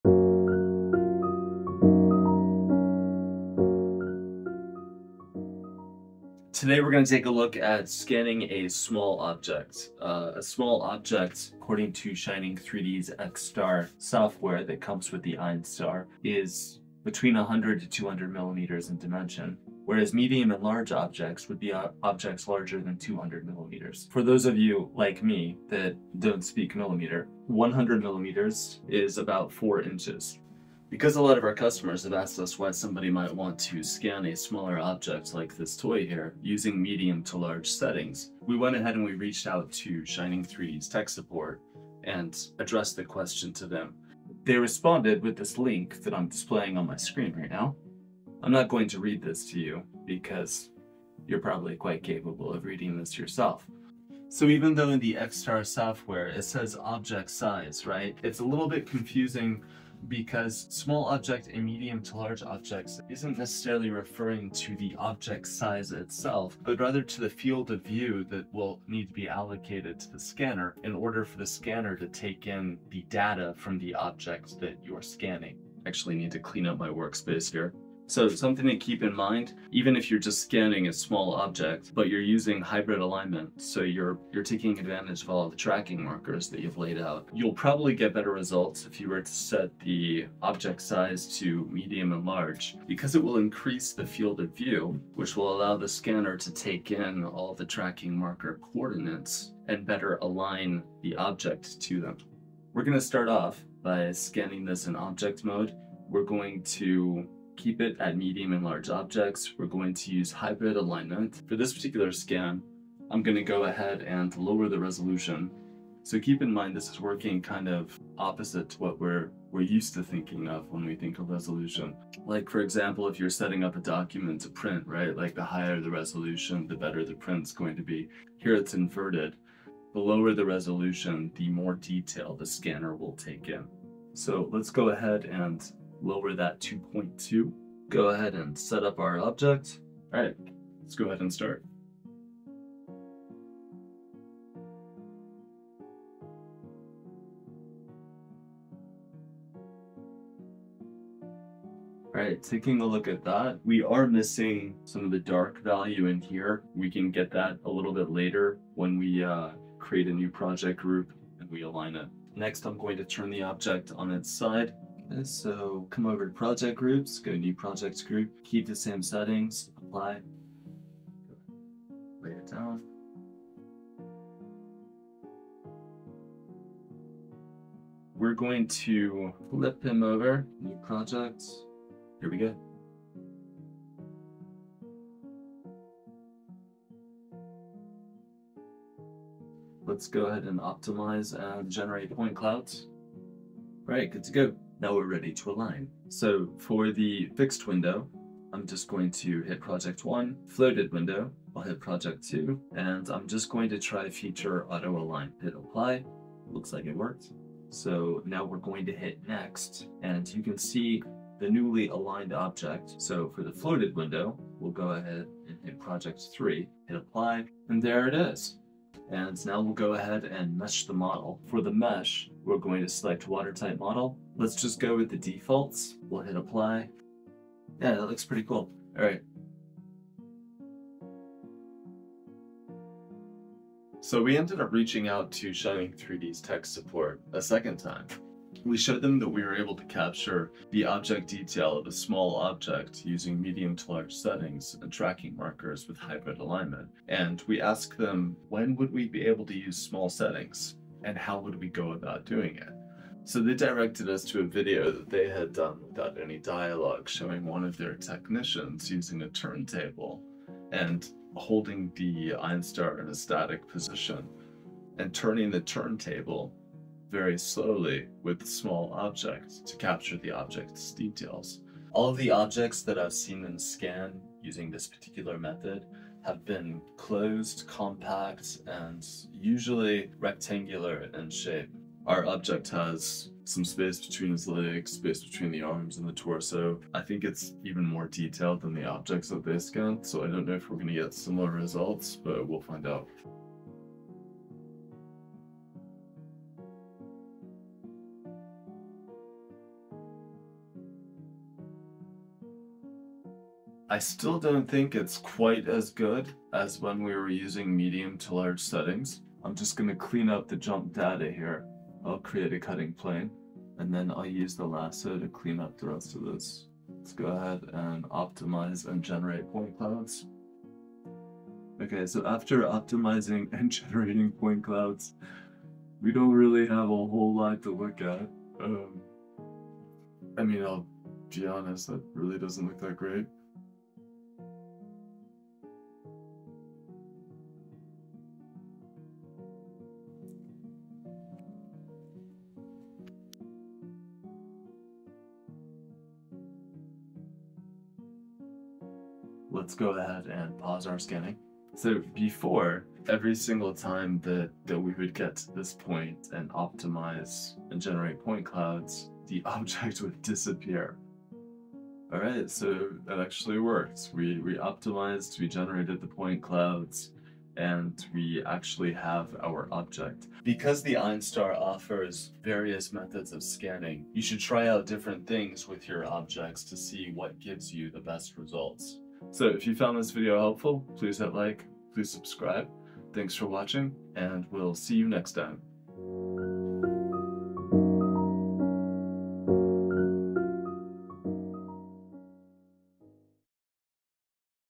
How to scan smaller objects with Einstar 3d scanner for better result?
Deep dive to explain how to use Einstar 3d scanner to scan small items for better result.